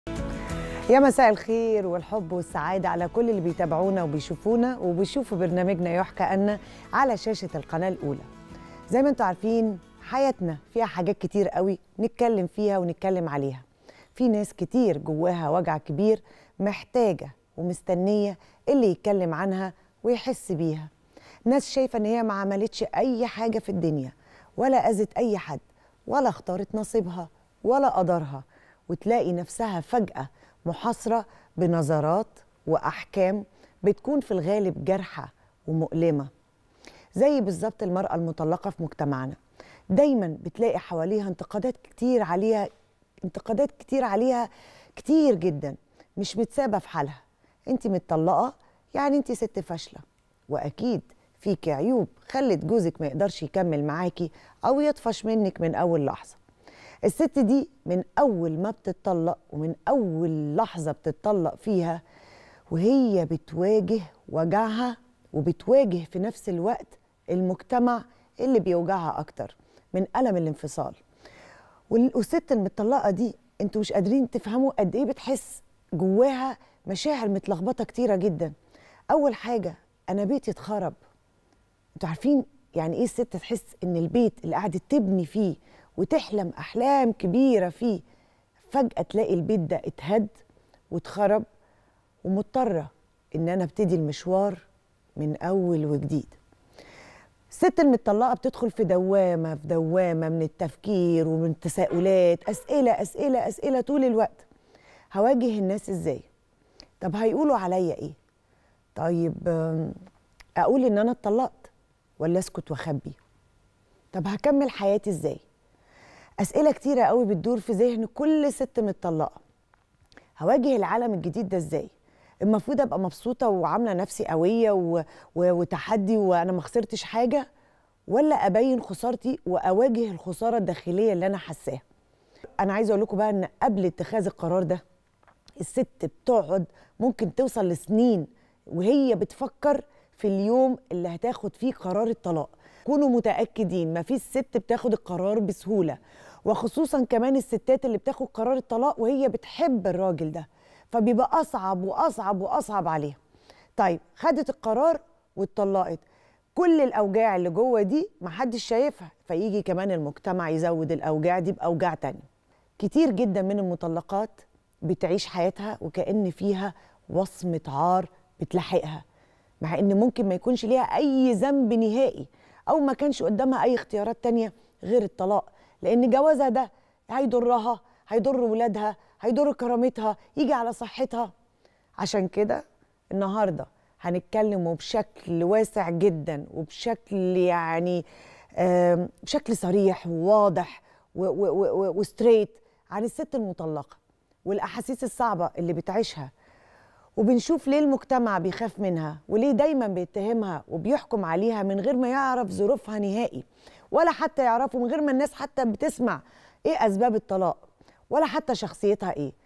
دول ناس شافو وجع كتير اوي في حياتهم ! .. مقدمة قوية من الإعلامية إيمان مختار عن" المرأة المطلقة "
دول ناس شافو وجع كتير اوي في حياتهم بس ياترى الغلط والعيب في مين ! .. مقدمة قوية من الإعلامية إيمان مختار عن " المرأة المطلقة "